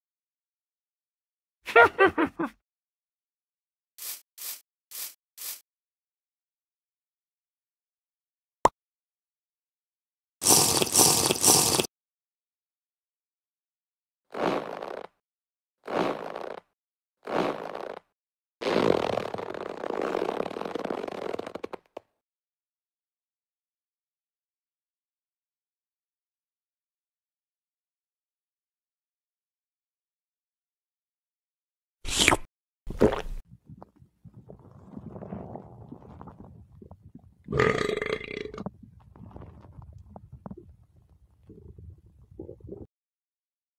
Minecraft Vore animation 4
This video is meant to be a continuation of the last video that had the witch where she tries out a potion she made.
I've been pretty drained on ideas so if you want you could give me an idea to motivate me to make more videos because it took a month for this idea to pop into my head.